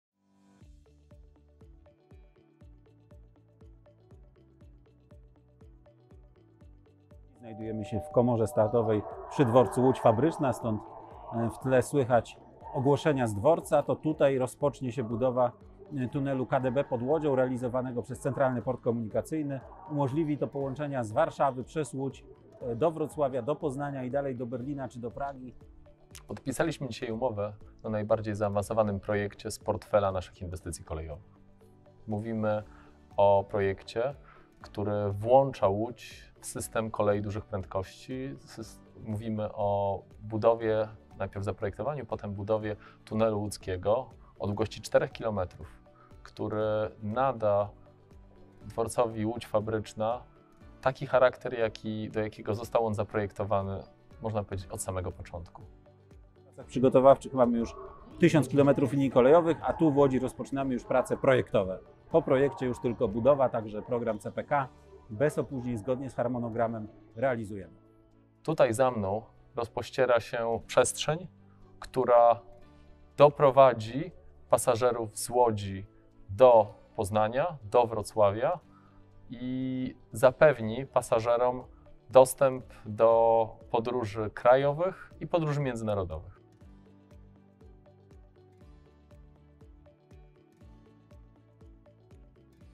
Tunel dalekobieżny w Łodzi - prace projektowe
Podpisanie umowy na zaprojektowanie tunelu dalekobieżnego w Łodzi